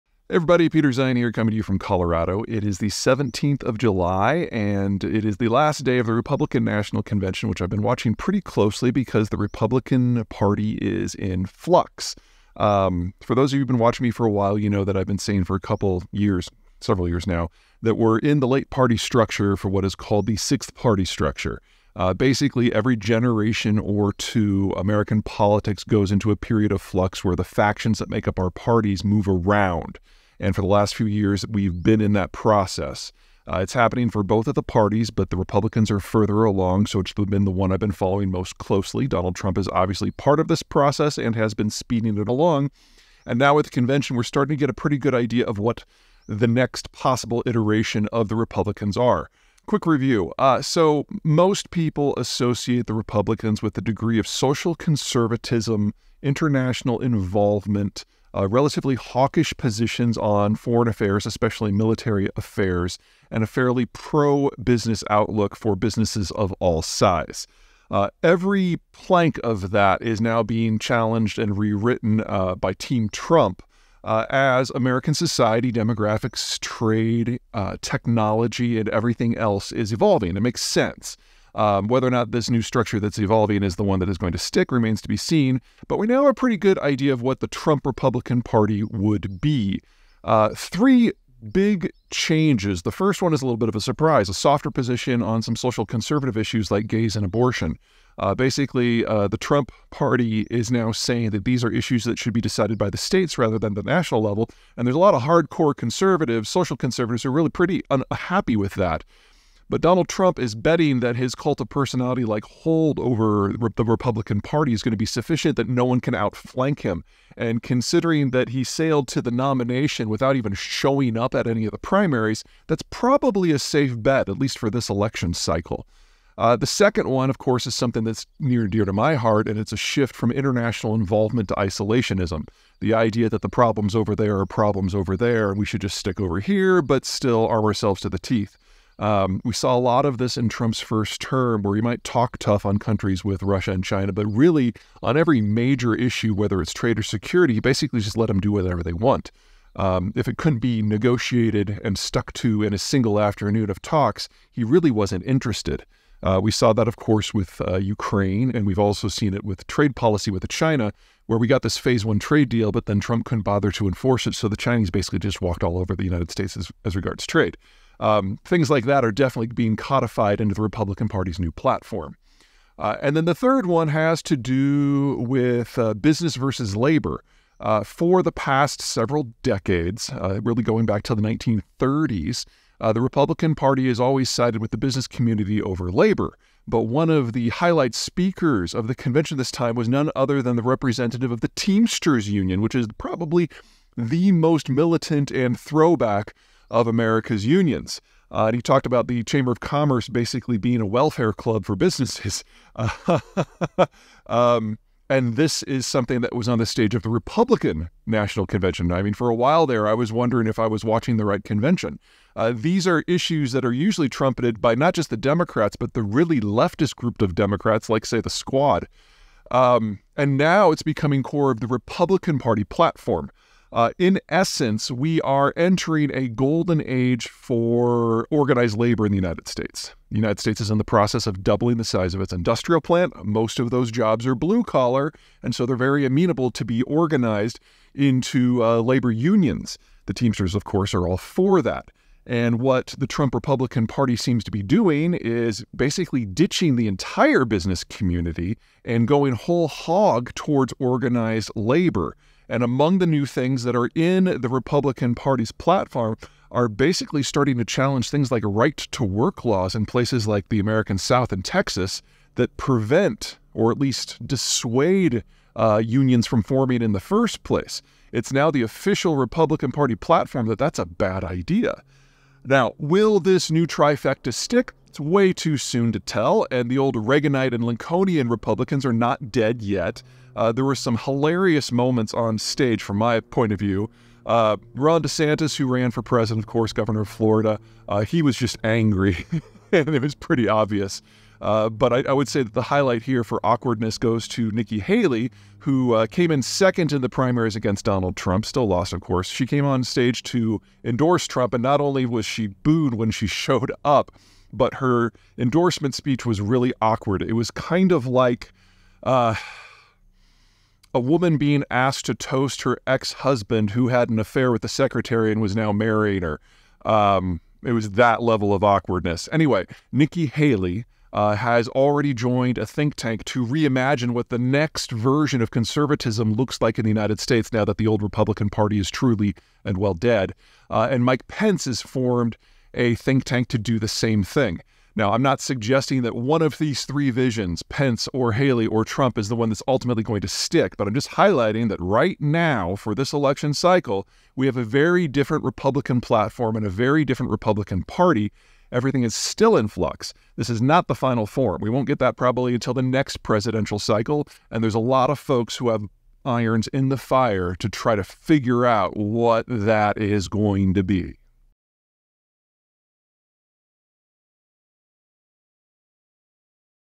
America’s Changing Republican Party || Peter Zeihan
The Republican Party is undergoing some evolutions at the moment, moving away from many of its traditional stances. This is part of a broader cycle in which American political parties shift and realign every few decades.